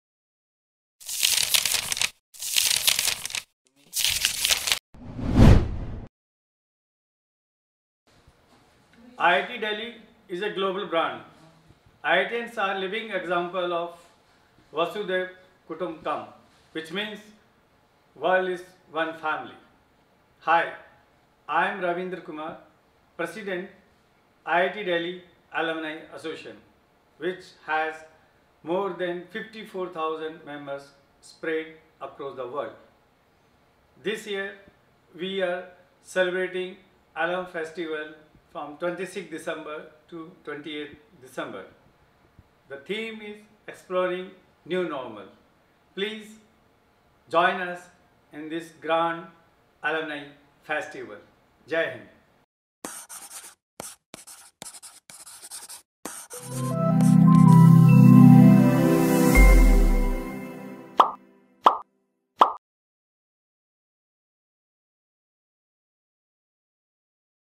Ravindra Kumar, President, IITDAA || IIT Delhi Alumni Fest 2020 "Exploring the New Normal" 26-28 Dec
IIT Delhi Alumni Association is celebrating  `Alumni Fest 2020’  on the theme of “Exploring the New Normal” (as part of Diamond Jubilee celebration of our alma mater) a flagship online three-days Conclave on 26-28 December 2020.
for the latest updates on the event.